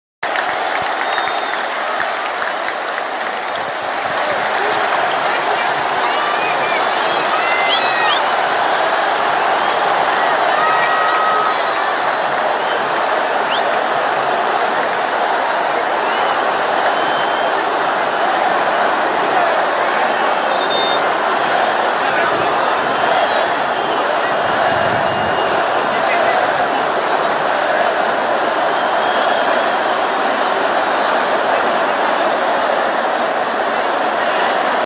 Torcida do Ceará, aplaudindo o golaço do Arraxca !
Puskás que pariu Arraxca . 🔴⚫